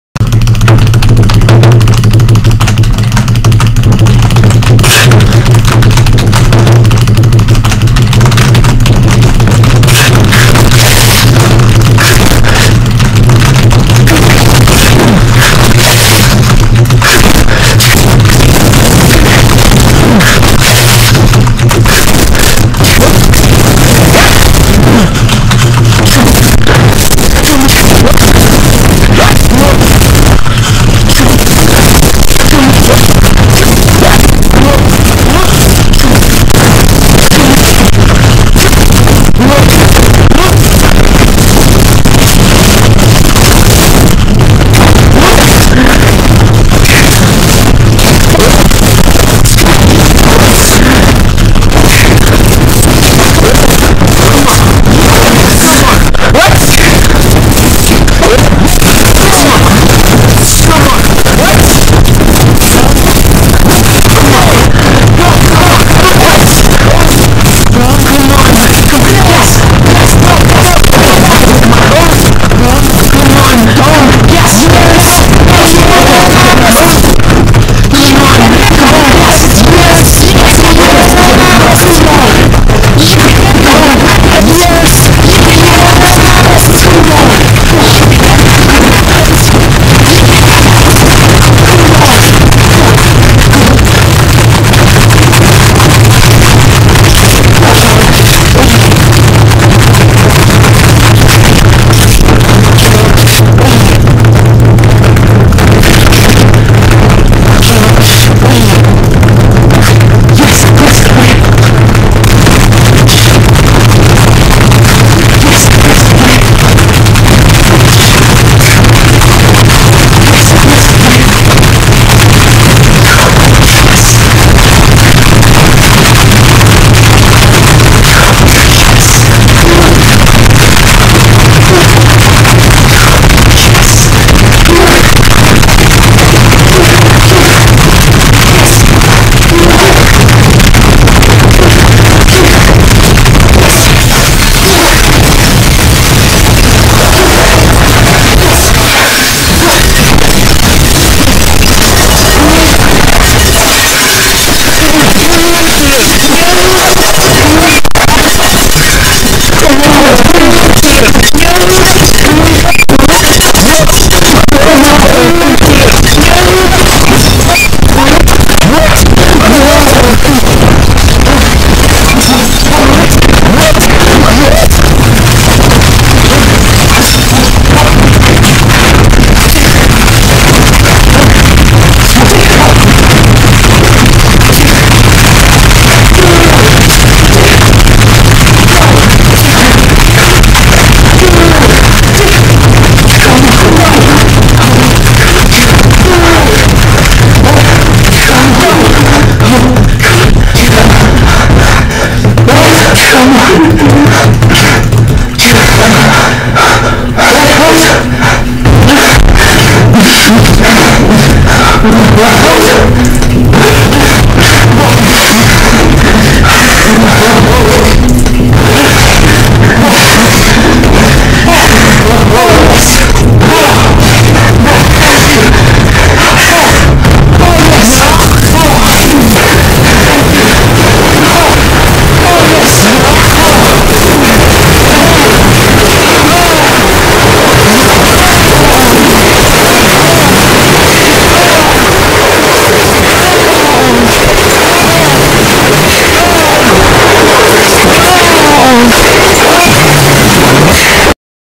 A Speedrunner Speedrunning A Speedrunner Speedrunning A Speedrunner
Today you're going to see a speedrunner speedrunning a speedrunner speedrunning a speedrunner. Yes. A totally ordinary Speedrunning meme.